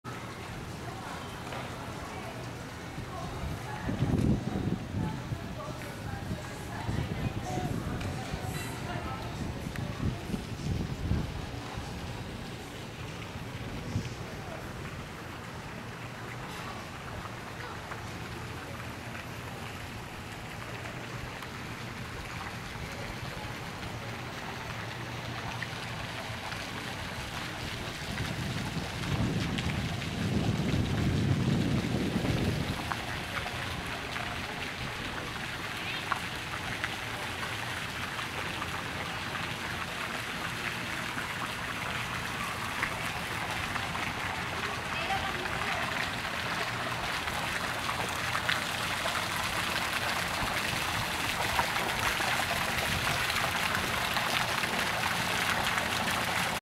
Anvaya Cove Beach & Nature Club, Morong, Bataan, Philippines (2)
Another nice look at the club and its fine facilities! Anvaya is really one of the best places to visit in Bataan, Philippines!